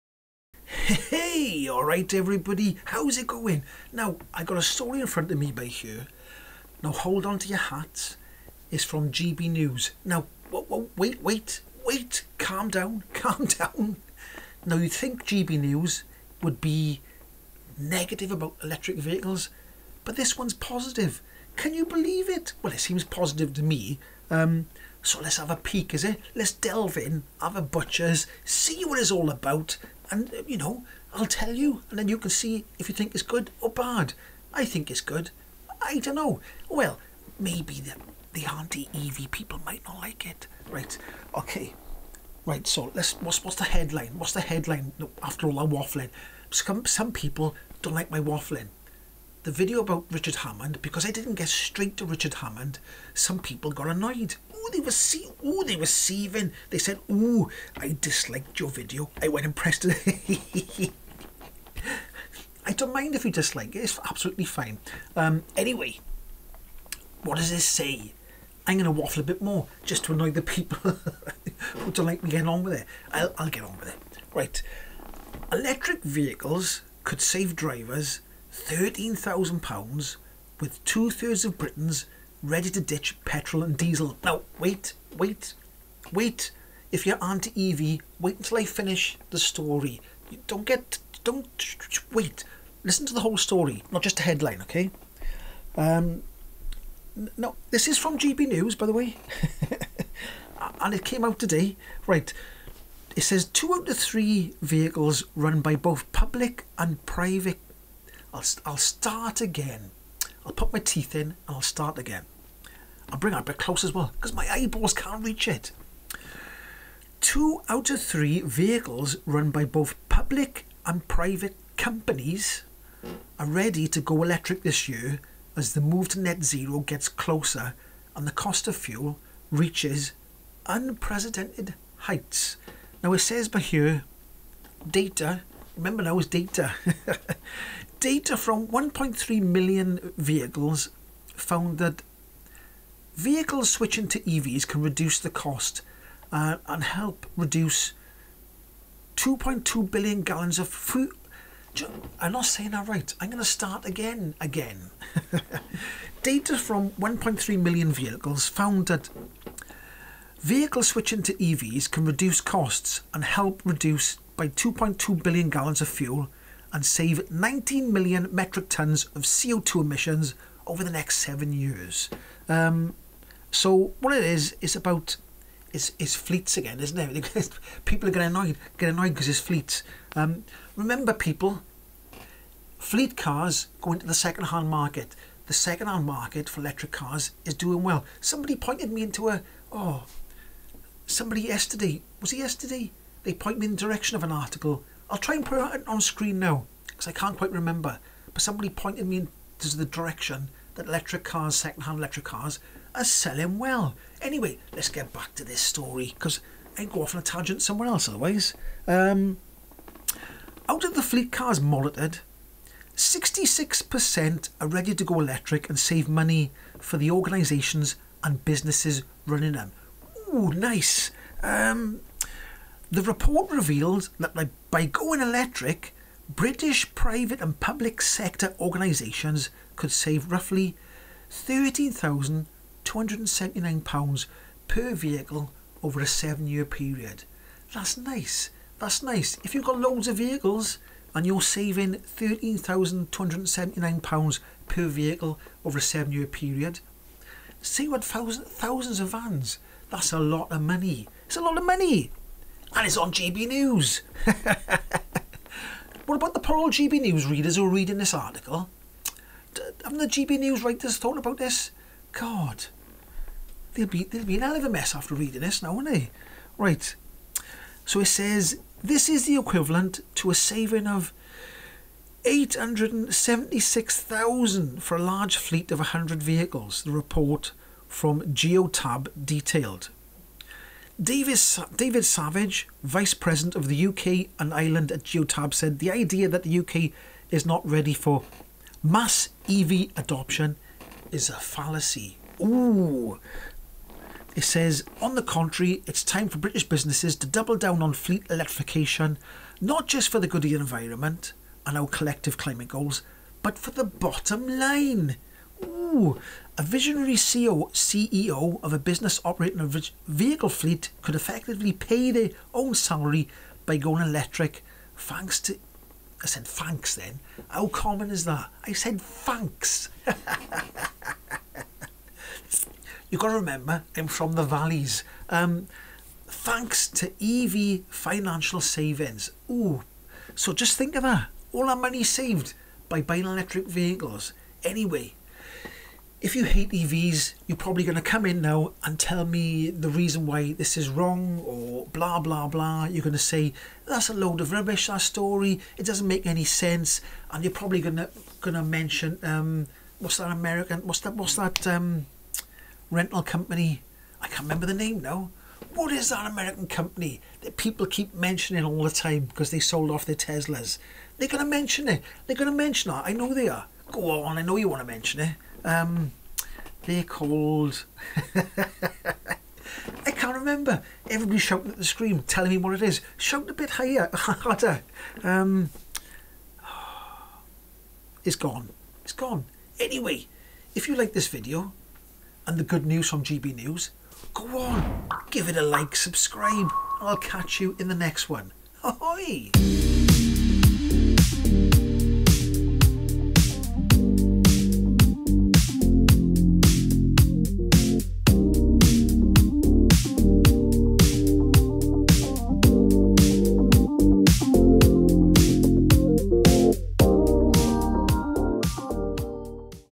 Electric vehicles could save drivers £13,000 with 66.6% of Britons ready to ditch petrol & diesel
Electric vehicles could save drivers £13,000 with two thirds of Britons ready to ditch petrol & diesel.

Data from 1.3 million vehicles found that vehicles switching to EVs can reduce costs, help reduce 2.2 billion gallons of fuel and save 19 million metric tons of CO2 emissions over the next seven years.

David Savage, vice president for the UK and Ireland at Geotab, said: “The idea that the UK is not ready for mass EV adoption is a fallacy.